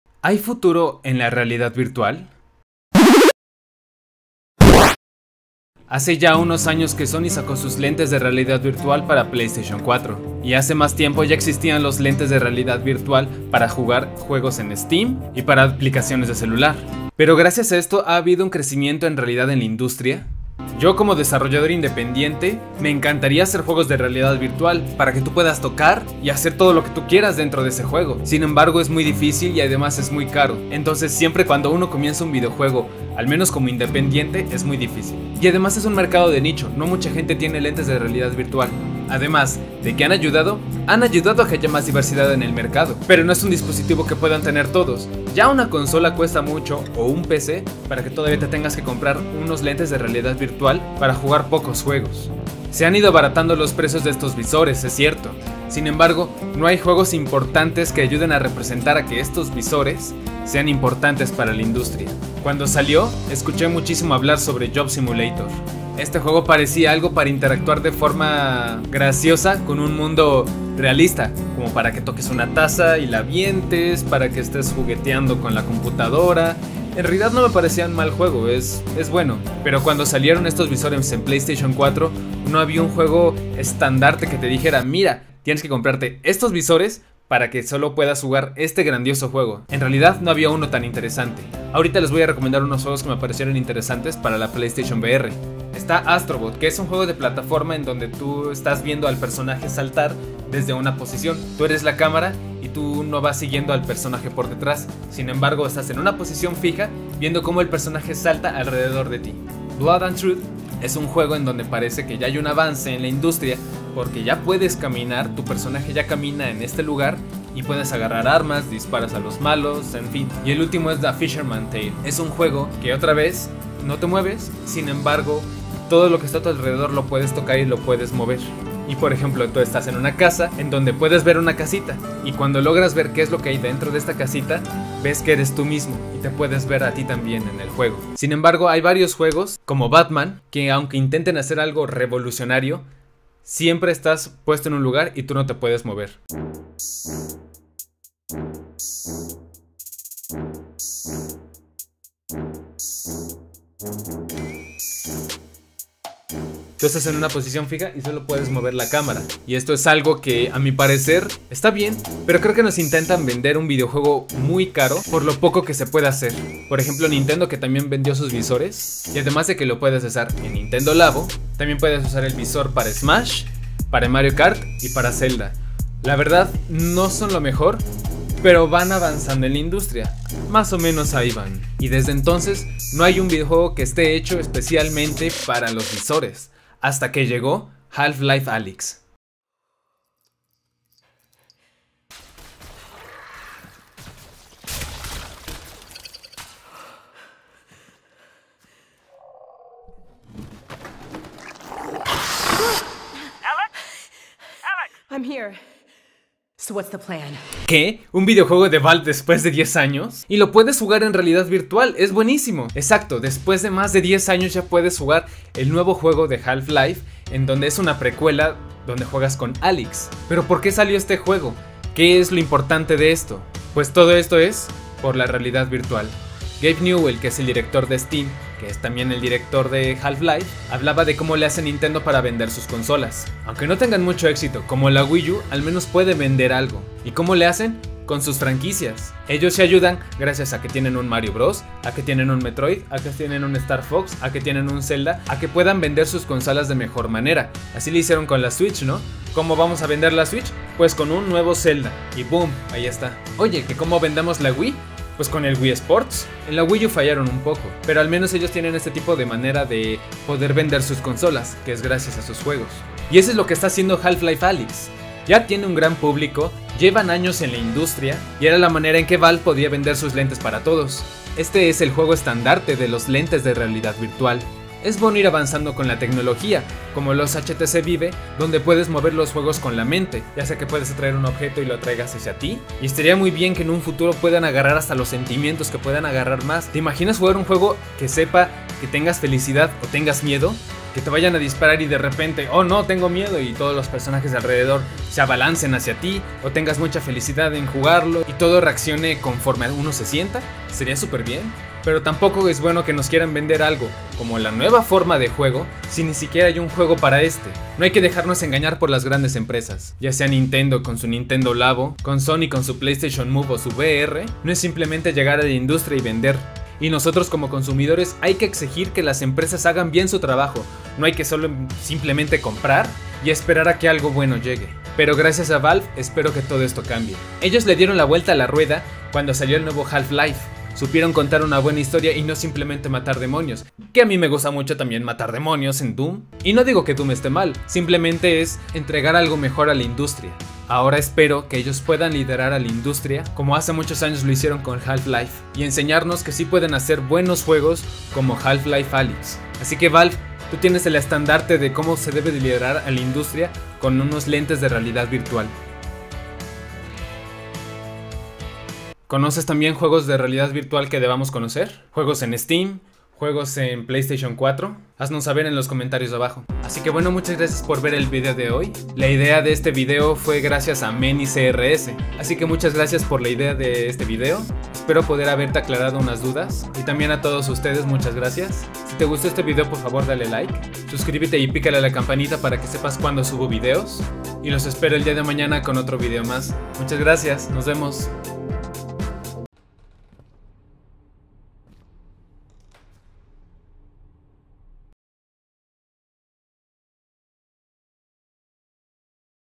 ¿Como la realidad virtual mejorará la industria? - Green Retroman
Hola chicos!! perdonen la hora a la que subo este video. ME tarde mucho porque hice mucha edición y esperaba que saliera mas rapido, espero que les guste, ya que este video es gracias a la idea de un subscriptor Mení Crs.
Gracias por seguirme y si quieren mas videos así no se les olvide comentarlo y lo hablamos!
My links!